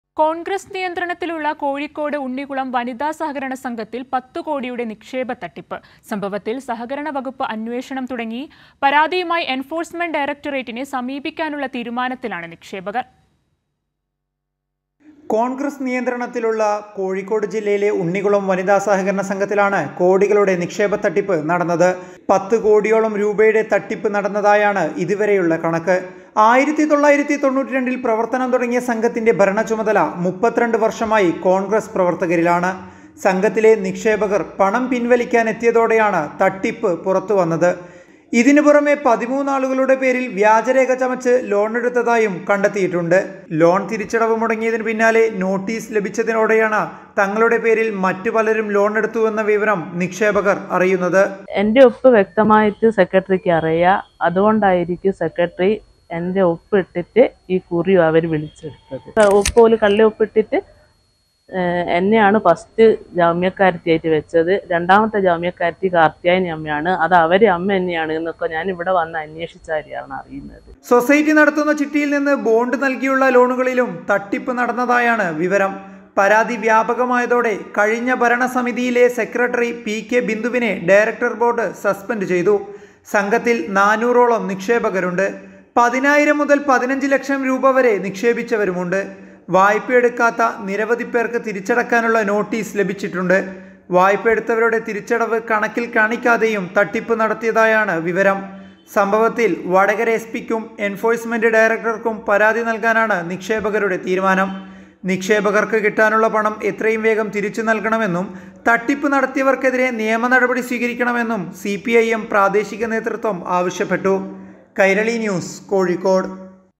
കോഴിക്കോട് ഉണ്ണികുളം വനിതാ സഹകരണ സംഘത്തിൽ 10 കോടിയുടെ നിക്ഷേപ തട്ടിപ്പ് | kozhikode | congress
കോൺഗ്രസ് നിയന്ത്രണത്തിലുള്ള കോഴിക്കോട് ഉണ്ണികുളം വനിതാ സഹകരണ സംഘത്തിൽ 10 കോടിയുടെ നിക്ഷേപ തട്ടിപ്പ്
10 crores investment fraud in Congress-controlled Kozhikode Unnikulam Women's Cooperative Society